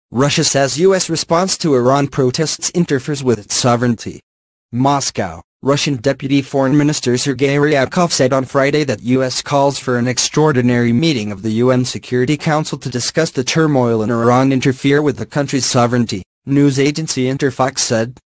RUSSIA SAYS U S RESPONSE TO IRAN PROTESTS INTERFERES WITH ITS SOVEREIGNTY
MOSCOW - Russian Deputy Foreign Minister Sergei Ryabkov said on Friday that US calls for an extraordinary meeting of the UN Security Council to discuss the turmoil in Iran interfere with the country's sovereignty, news agency Interfax said.

The deputy minister added that Tehran's statements that external influences fomented the protests were not groundless and that the United States uses any possible method to destabilize governments it dislikes.